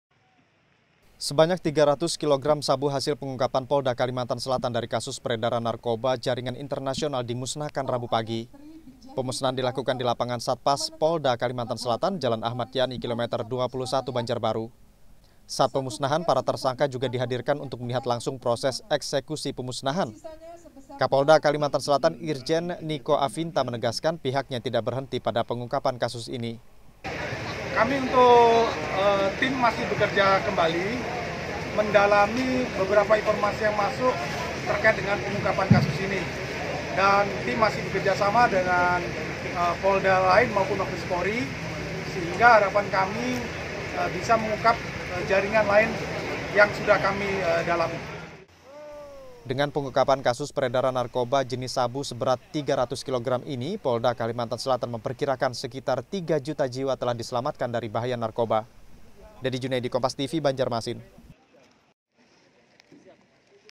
Musnahkan 300 Kg Sabu, Polda Kalsel : Sekitar 3 Juta Jiwa Diselamatkan
BANJARBARU, KOMPAS.TV - Sebanyak 300 kilogram sabu hasil pengungkapan   polda kalsel dari kasus peredaran narkoba jaringan internasional   dimusnahkan, rabu pagi (2/9/2020).

 pemusnahan dilakukan di lapangan  satpas polda kalsel, jalan ahmad yani kilometer 21, Banjarbaru.
Saat pemusnahan, para tersangka   juga dihadirkan untuk melihat langsung proses eksekusi pemusnahan.

Kapolda Kalsel Irjen Nico Afinta  menegaskan pihaknya tidak  berhenti pada pengungkapan kasus ini.
Dengan  pengungkapan kasus peredaran narkoba jenis sabu seberat 300 kilogram ini, Polda Kalsel memperkirakan sekitar 3 juta jiwa telah diselamatkan    dari bahaya narkoba.